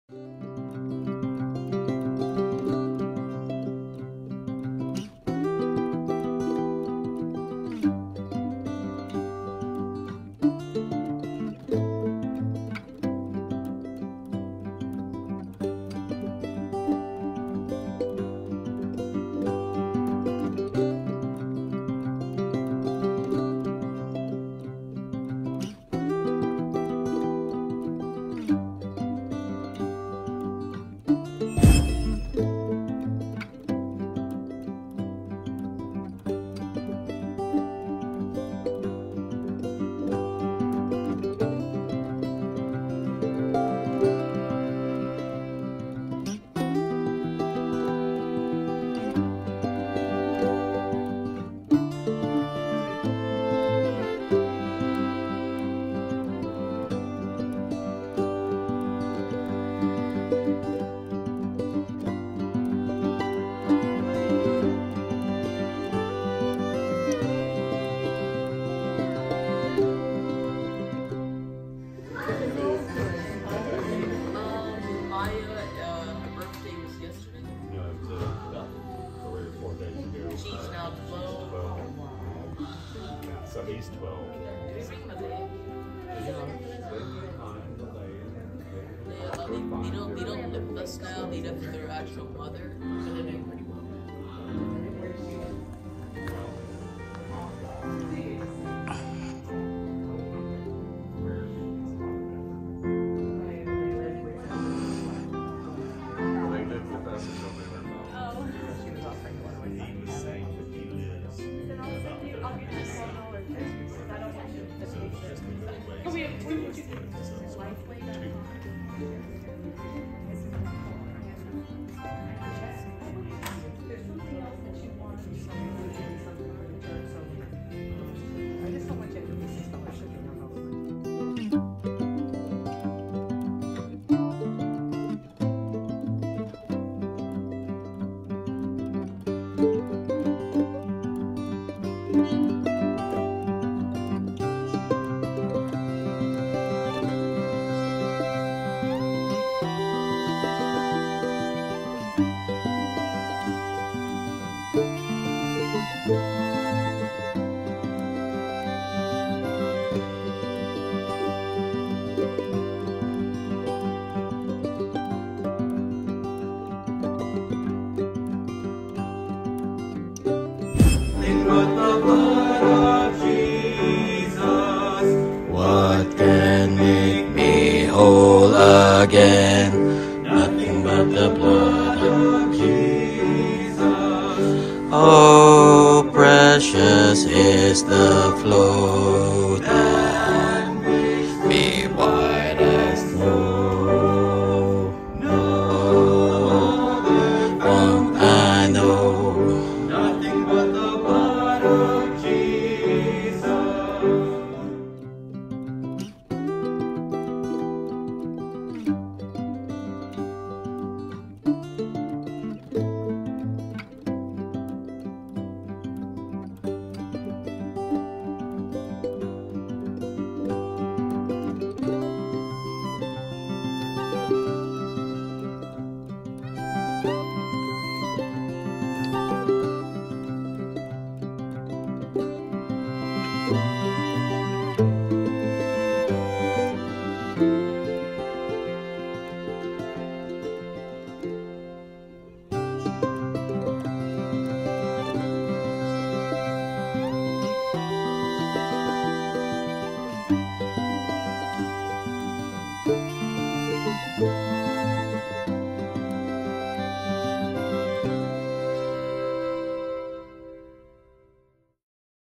2K23 July 2, Church activities: preparation for a service.
Jon and Sarah visits with their kids, too!